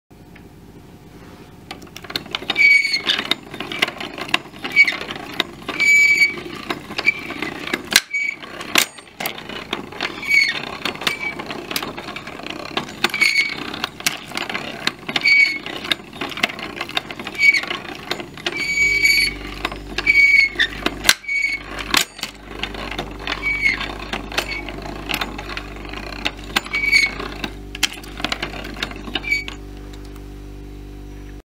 fainting goat movie
A Fainting Goat eating fire crackers.